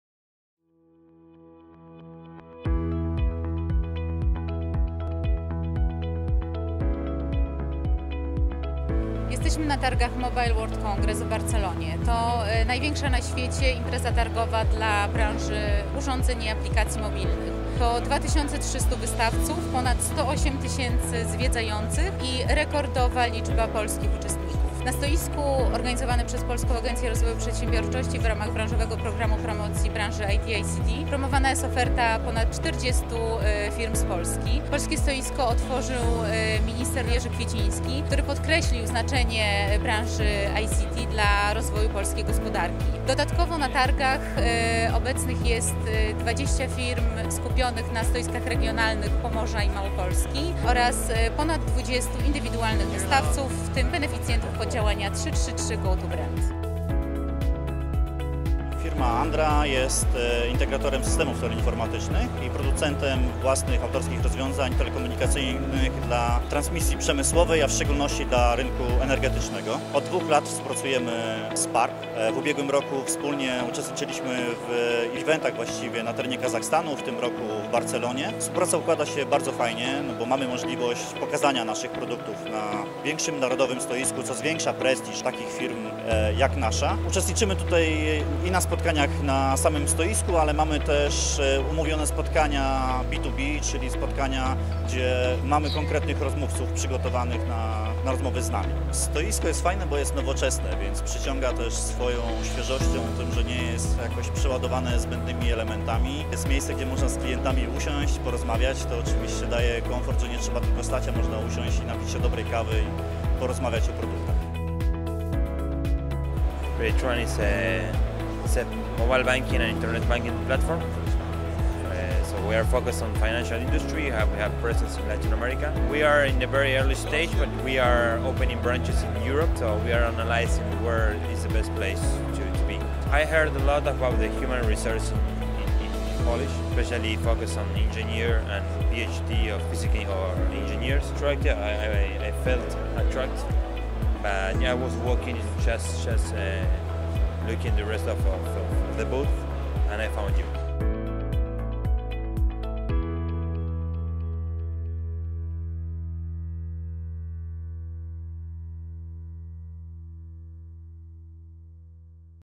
Polish ICT promotion stand at Mobile World Congress 2018 in Barcelona (Spain)
On February 26 - March 1, 2018 the Polish Agency for Enterprise Development organized a promotional stand at the Mobile World Congress in Barcelona (Spain). During the show 68 Polish companies had an opportunity to promote their offer at the Polish ICT Promotion Program stand. More about the program